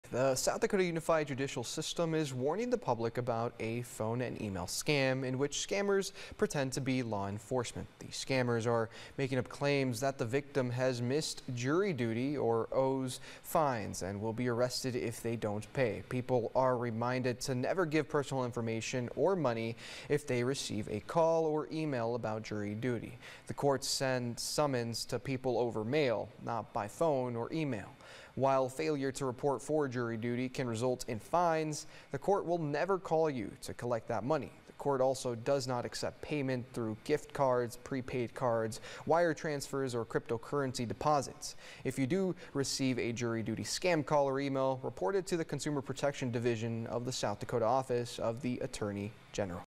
Urgent warning: New jury duty scam targeting residents
Scammers are making up claims that the victim has missed jury duty or owes fines and will be arrested if they don’t pay.